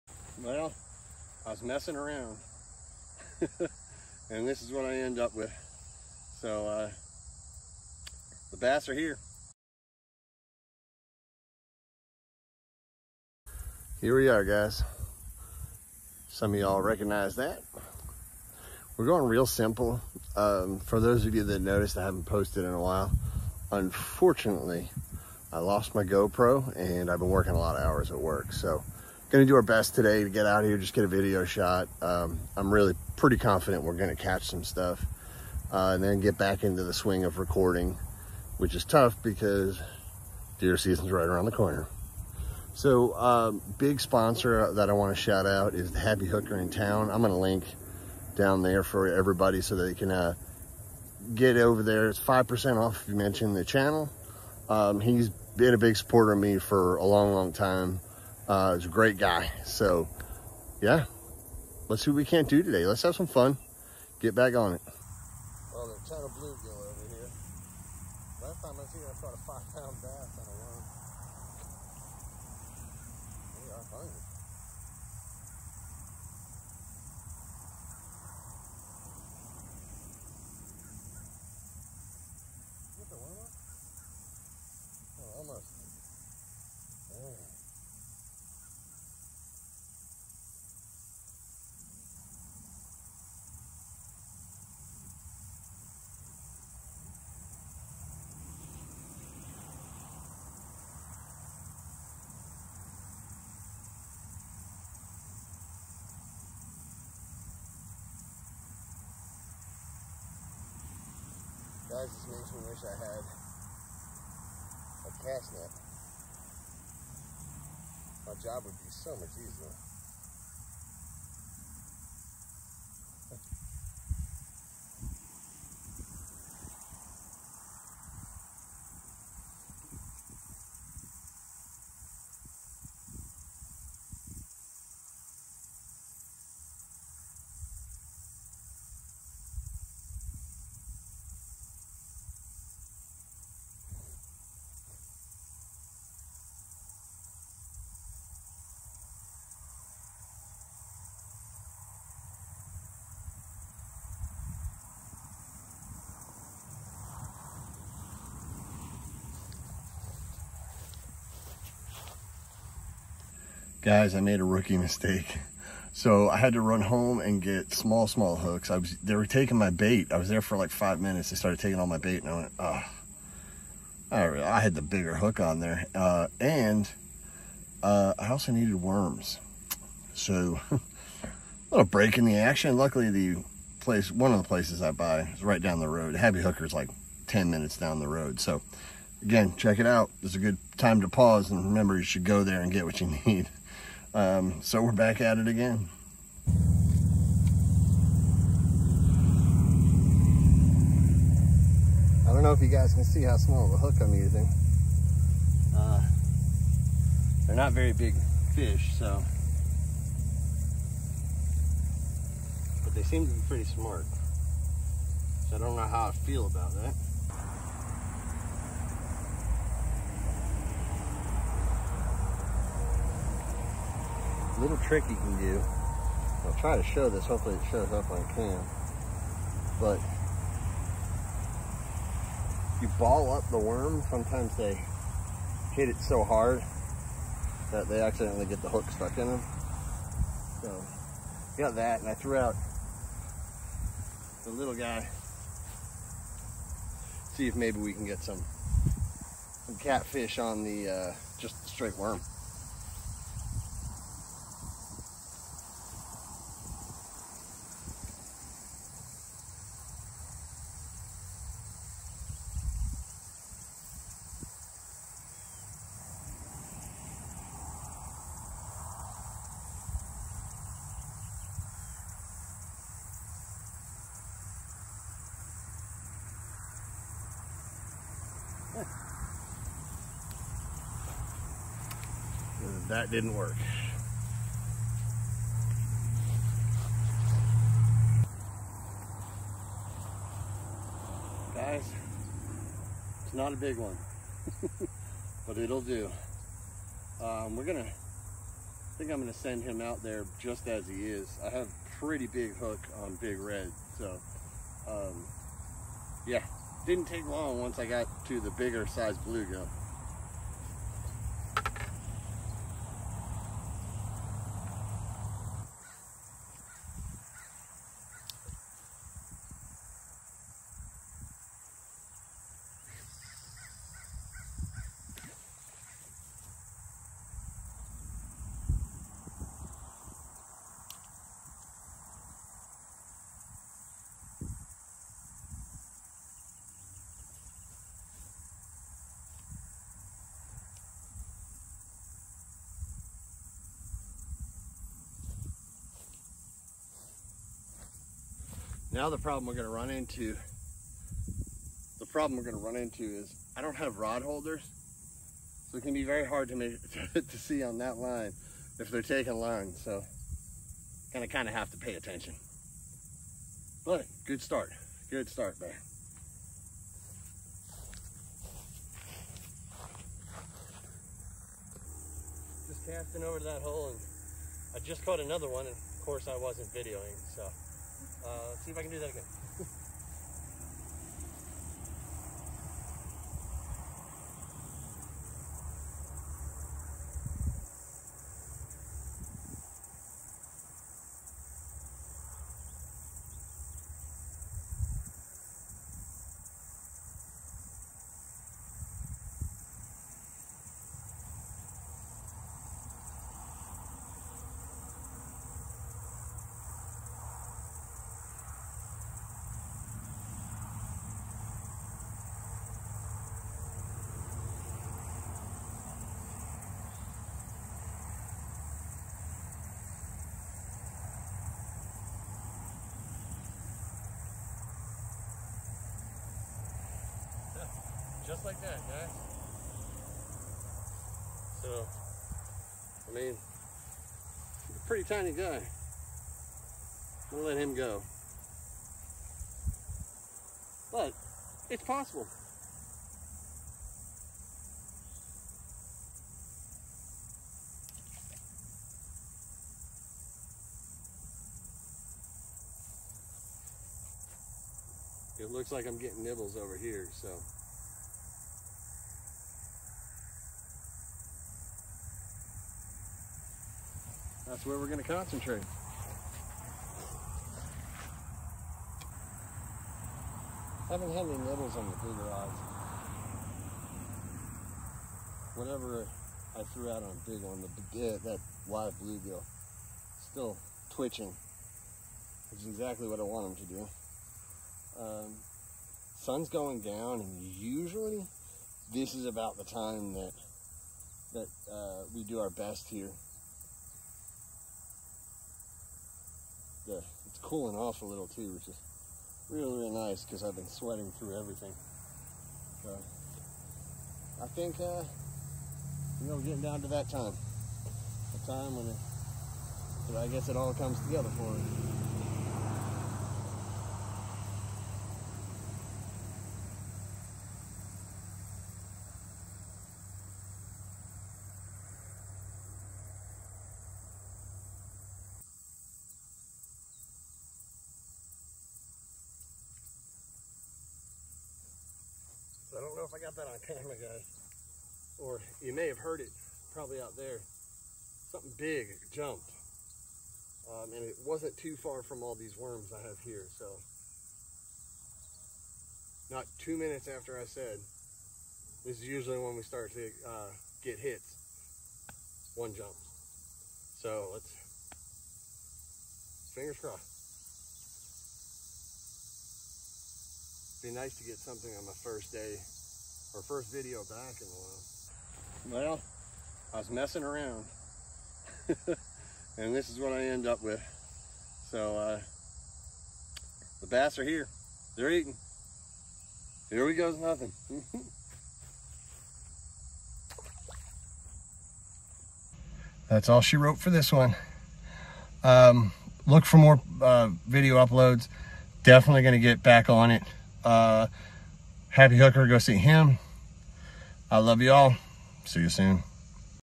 Im back AGAIN AGAIN!! Its cooling off and Im out fishing again. The fishing is heating up.
Its been far too hot for a cancer surviver to be fishing. I lost my GoPro which did not help. Ive also been working a lot. Some nice catches and it felt good to feel the pull on the line.